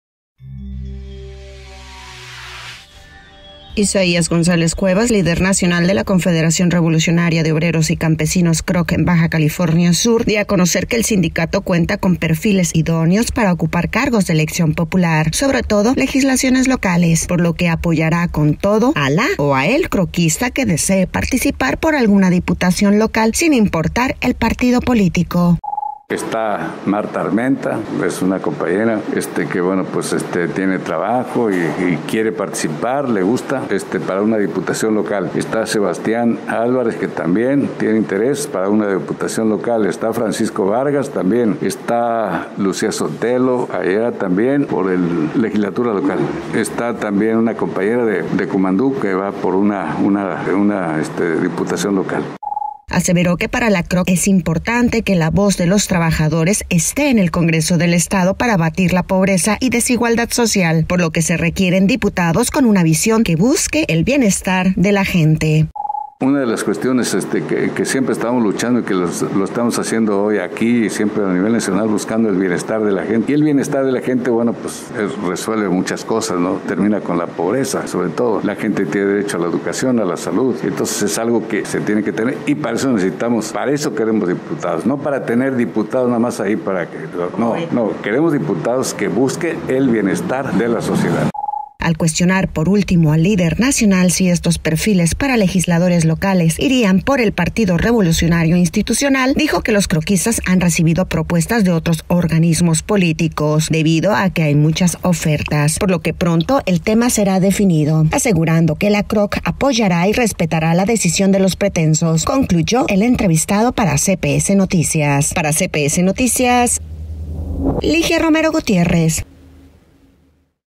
La CROC tiene perfiles para diputados locales
La CROC tiene perfiles para diputados locales, definirán por qué partido, informó su dirigente nacional, Isaías González.